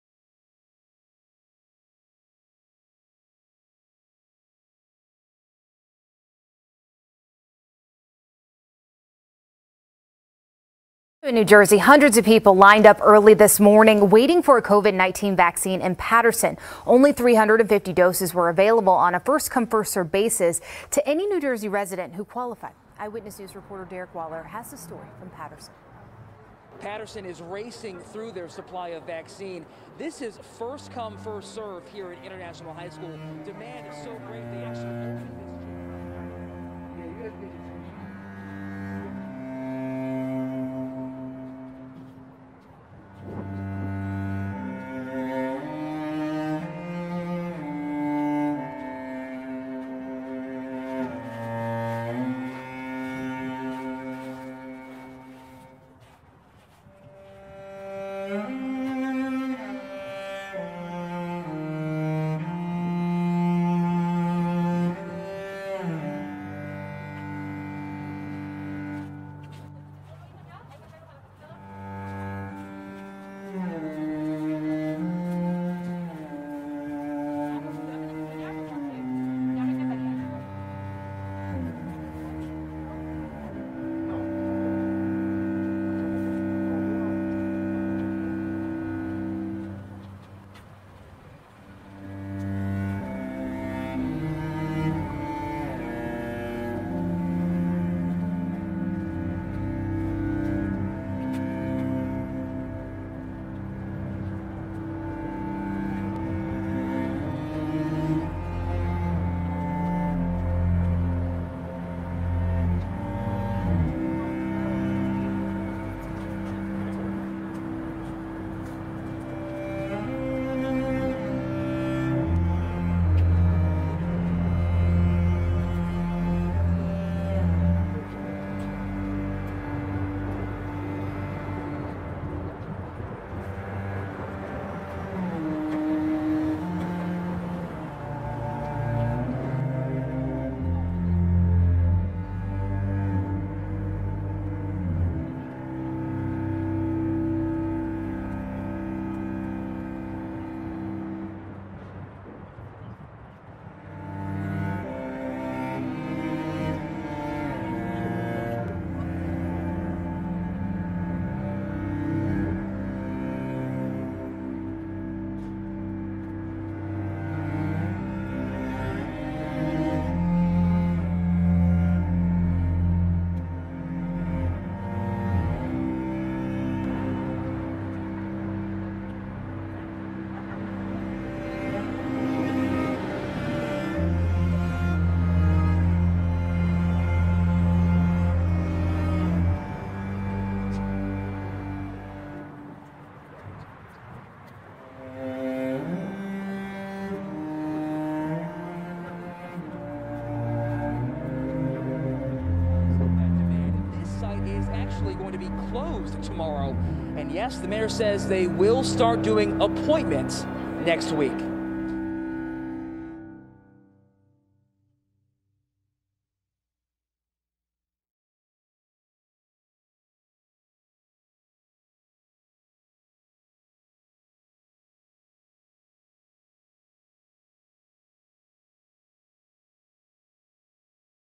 The Covid Games
Our nation, preeminent in science, medicine and resources, mismanaged the threat of COVID-19—preparedness and response, equipment and protective materials, testing and tracing. We continue to stumble in administering an effective production strategy, national distribution network and infrastructure for vaccination. The hardship characterized in this film belies the greater frustration in following prescribed registration, seeking an appointment and achieving a semblance of protection from the pandemic.

The haunting resonance of Aimee Norris’ cello serves to underscore the lengths to which so many strove to attain salvation from the scourge. I offer my work in tribute to those who waited and to the 350 who were rewarded for their labors.